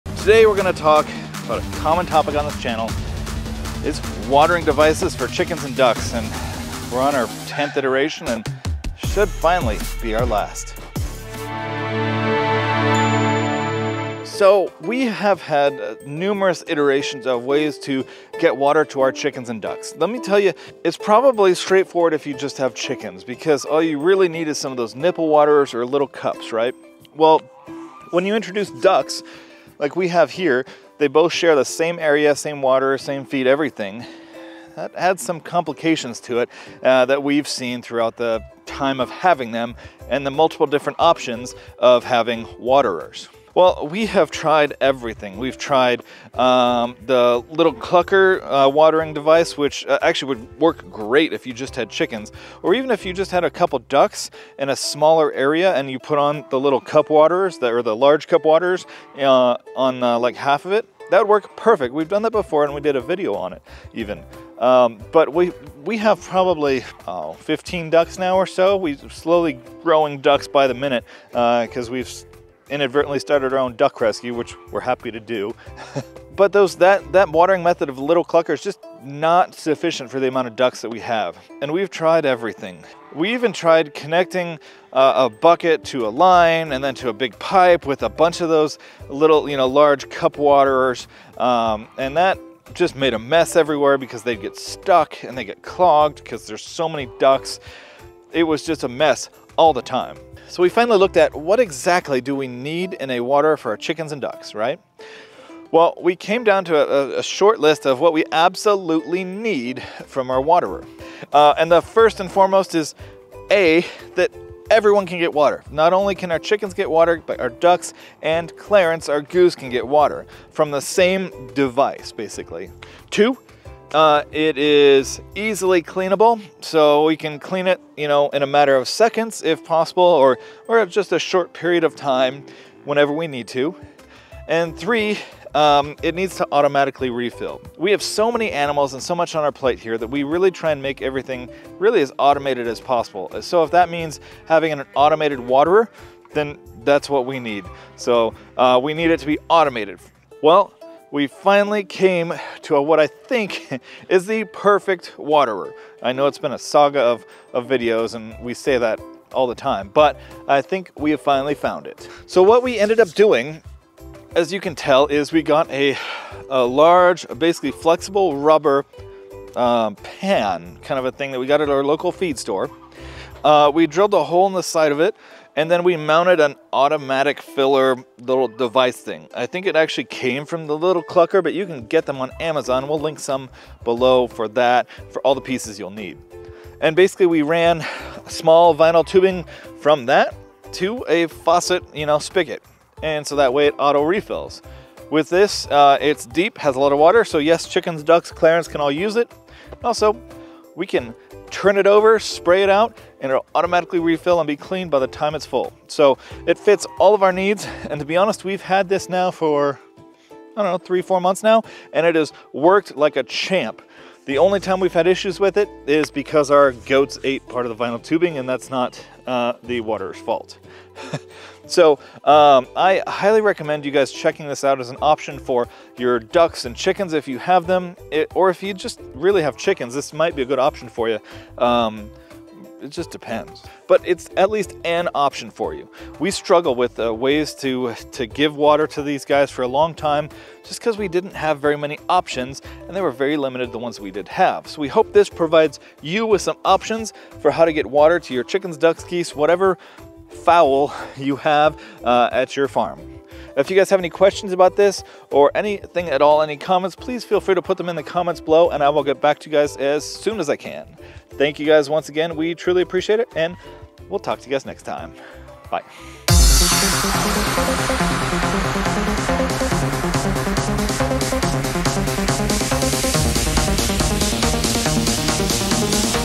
The LAST poultry waterer you'll ever need - BUYERS GUIDE
Say goodbye to constantly replacing poultry waterers! In this video, we review the last waterer you'll ever need for your chickens and ducks on your homestead or family farm. No more leaks or spills - this waterer is a game-changer!

Lil Clucker Waterer
Large Cup Waterers
Rubber Pan
Float valve